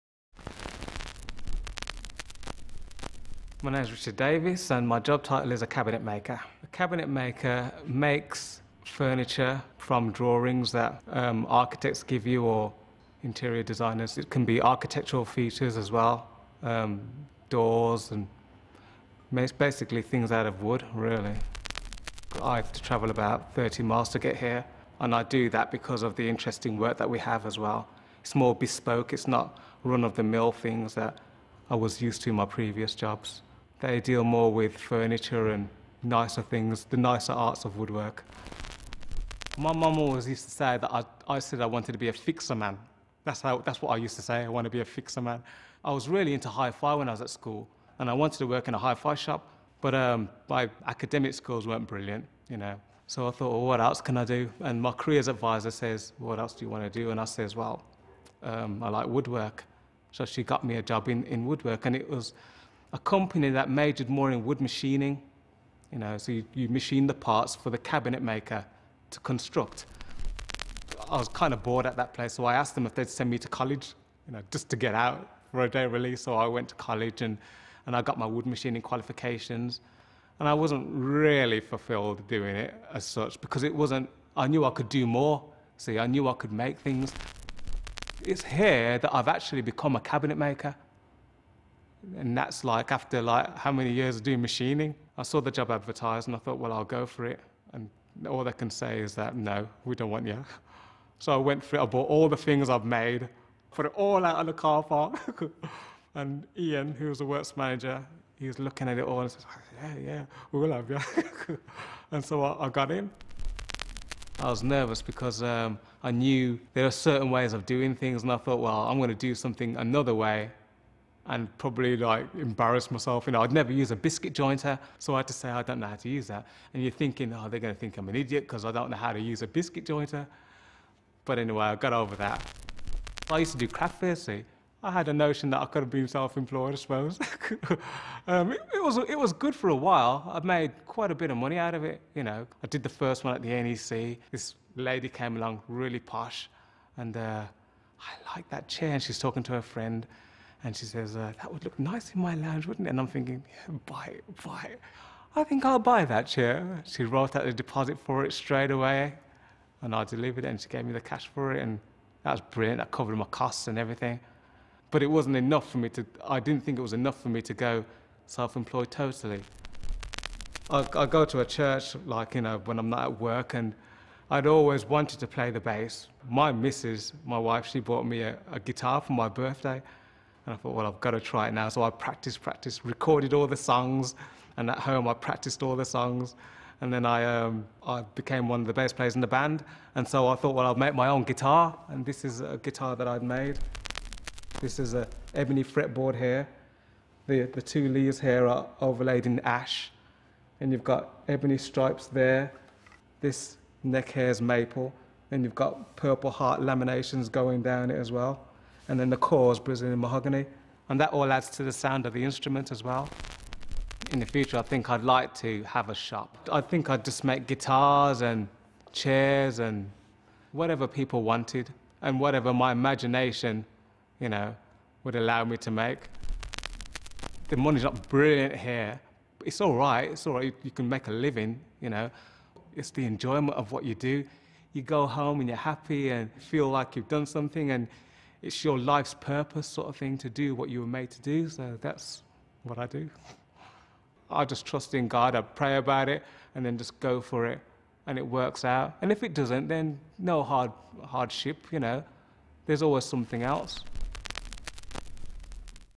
Career Advice on becoming a Cabinet Maker by Richard D (Full Version)
for more careers info.

Ricahrd Davis is a Cabinet Maker with NEJ Stephenson. Starting out with few qualifications as a wood machinist, Richard's drive, enthusiasm and creativity has got him a job as a cabinet maker. He also designs and makes guitars and furniture as a hobby. He goes home happy and says "you feel like you've done something and it's your life's purpose". His mum always said he should be a "fixer man".Highlights